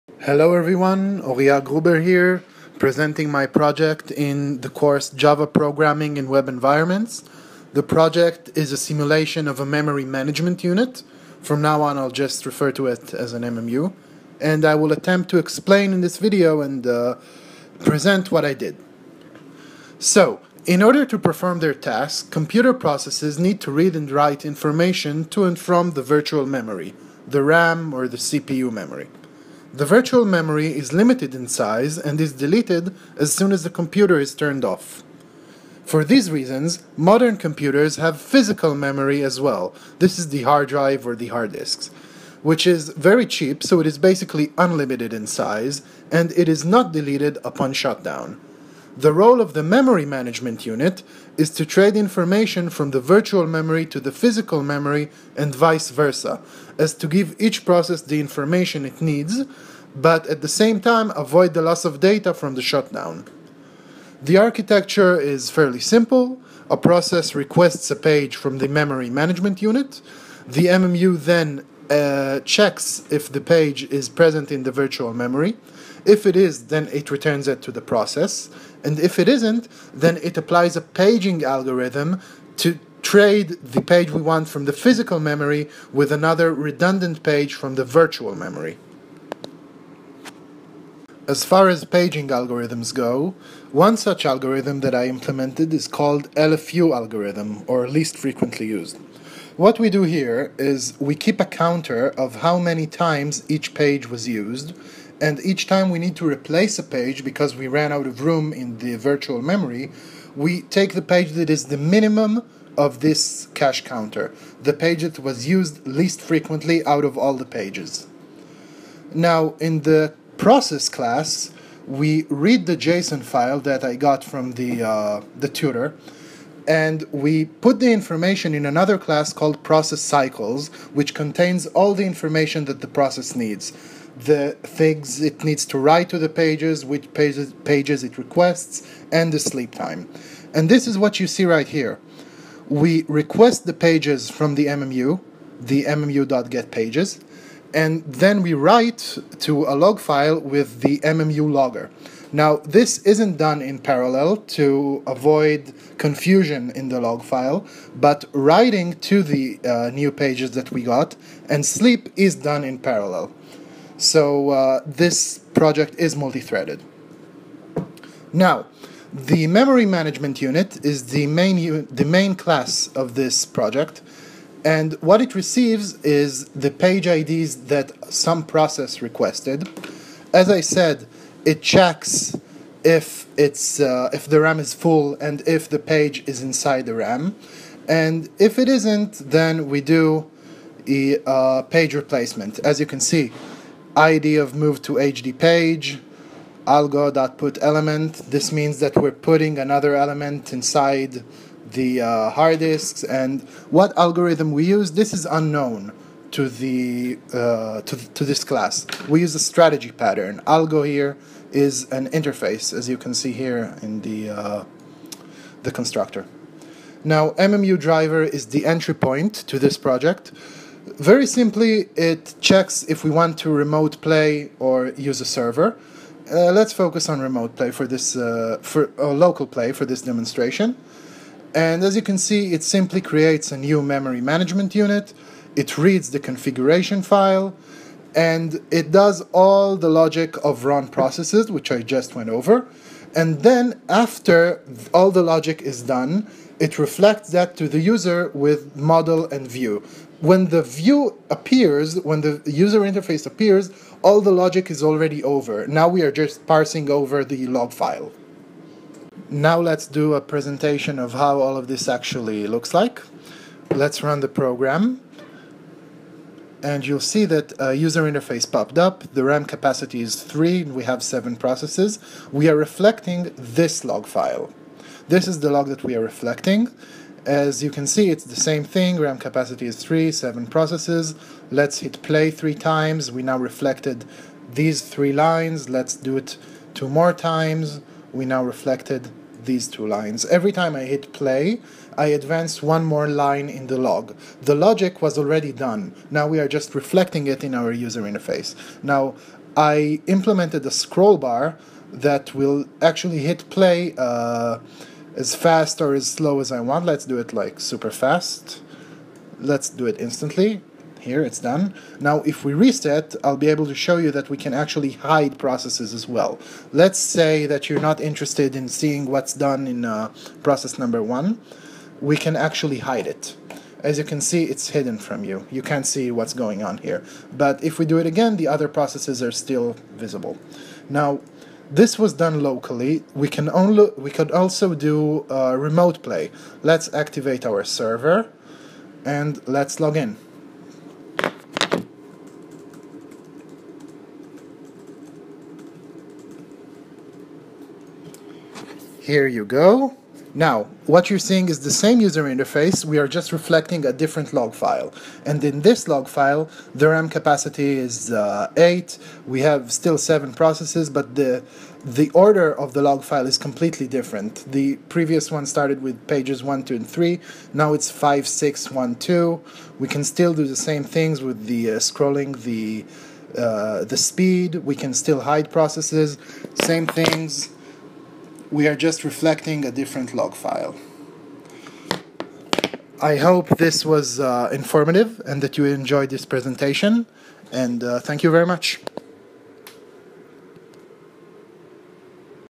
Java MMU Project Presentation
A presentation of my project in the course Java programming in a web environment.